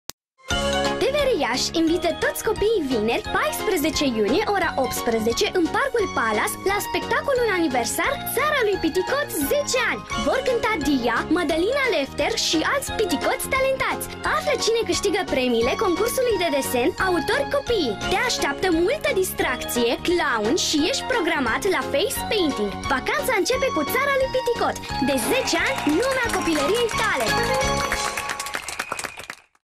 Țara lui Piticot - spectacol aniversar 10 ani
Țara lui Piticot se pregătește de petrecere! Vineri, 14 iunie, de la ora 18.00 Sfetnicul Știe-Tot vă așteaptă în parcul Palas la spectacolul aniversar de 10 ani Țara lui Piticot!

Micii moderatori au pregătit surprize pentru toți copiii: face-painting, picioroange și clovni care să înveselească atmosfera. Dya și Mădălina Lefter au pregătit muzica și... restul surprizelor le descoperiți vineri la Palas Iași!